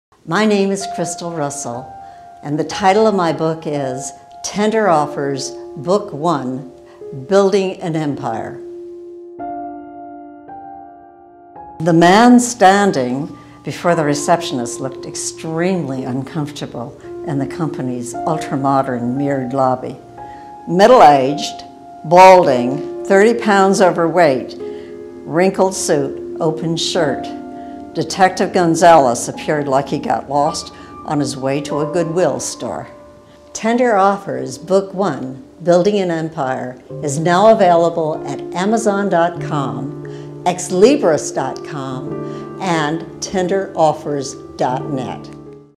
Tender Offers by Crystal Russell - What's your favorite passage? - 3rd part
This book is the first of a two book series. Set in Beverly Hill, California it is the story of a onetime B actor, Stanford Wentz, who forms an unlikely partnership with his unprincipled agent, Aaron Rose, to build the Stanford Wentz Cosmetics Company. The entertainment and business worlds collide in a cut throat industry where beauty is marketed like a commodity. Through unscrupulous and ruthless deals, the company is propelled to the limelight of the Wall Street by Aaron Rose, the driving force who is willfully cold-blooded. Stanford Wentz, willfully ignorant, goes along for the ride. The curtain is lifted to expose the hype of the slick world of advertising where sales, image and consumer perception are the only things that matter. Set in the artificial world of beauty, the corporate executives are willing to do anything for money. It is a fast moving suspense thriller, filled with corporate politics, intrigue, legal maneuverings, litigation and murder to show the world behind the image. Most of all, it is a lawyer story. It shows how corporate lawyers become willing and able assistants in the rule bending dictated by business necessity. It is the story about building a cosmetics empire, and about money, greed and unbridled ambition.